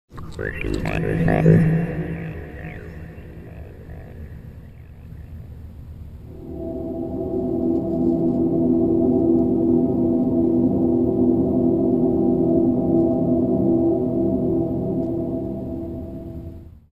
Creepy Thing
Found this on the bathroom floor late at night, creepy!